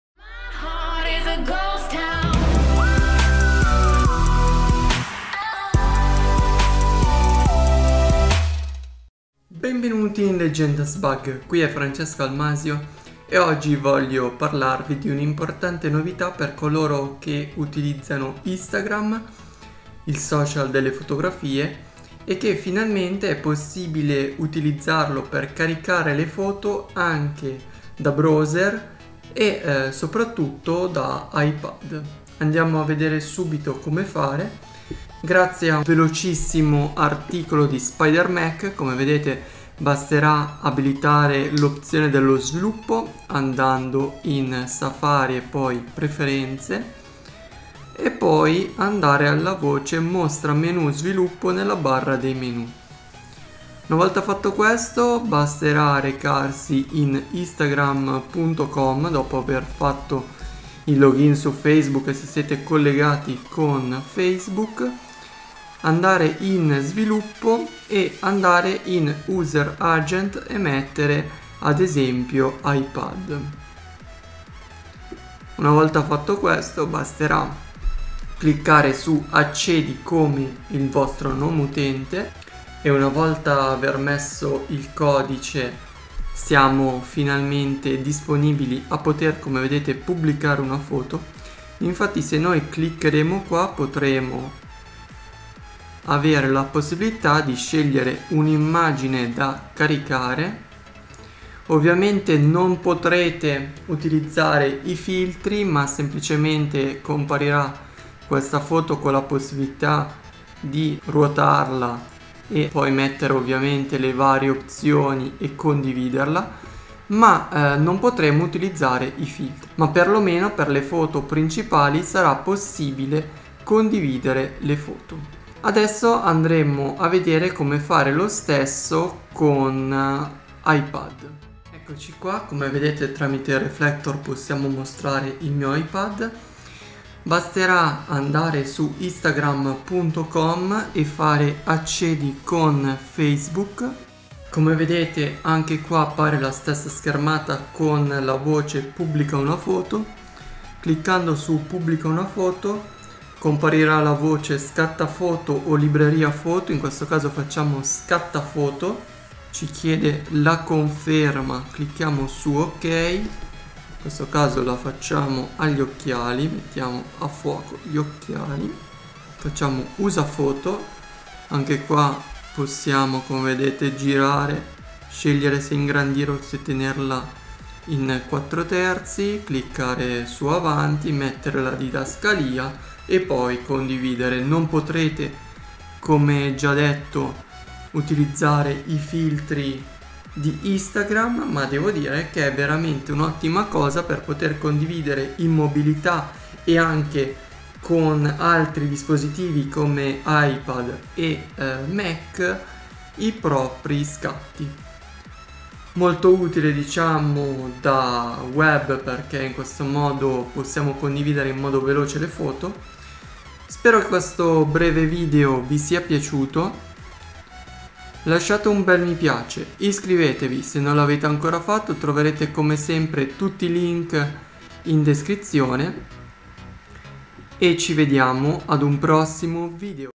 Pubblicare foto su Instagram da Mac e iPad!
Tutorial in cui vi mostro come poter caricare le foto su Instagram, il social delle foto, direttamente da Safari per Mac e iPad senza la possibilità di abilitare filtri.

Info personali

Quando pubblico?
VIDEO: Lunedì, mercoledì e venerdì alle 14:30. Domenica alle 14.
LIVE GAMING Ottimizzate con link attività alle 17.